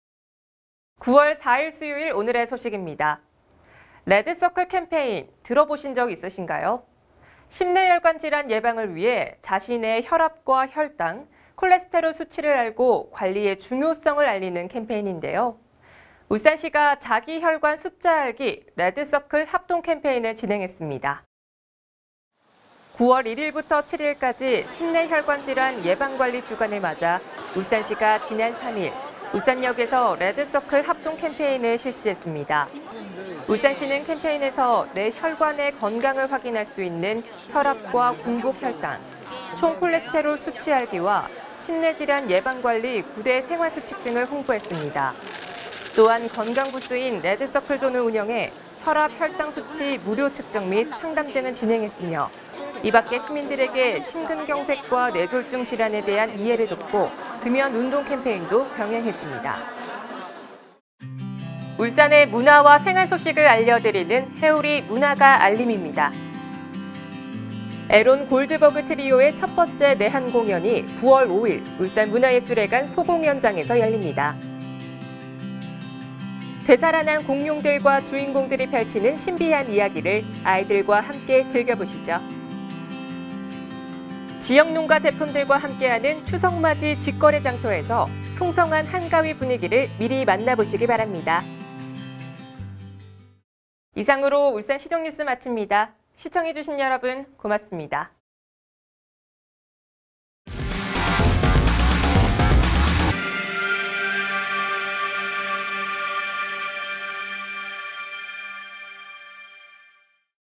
[2019년 9월 4일(수) 오늘의 뉴스]
1. 자기 혈관 숫자 알기, 레드서클 캠페인
9월 1일부터 7일까지 심뇌혈관 질환 예방관리 주간을 맞아 울산시가 지난 3일 울산역에서 ‘레드서클 합동 캠페인’을 실시했습니다.
울산시는 캠페인에서 내 혈관의 건강을 확인할 수 있는 혈압, 공복혈당, 총 콜레스테롤 수치 알기, 심뇌질환 예방관리 9대 생활수칙 등을 홍보했습니다.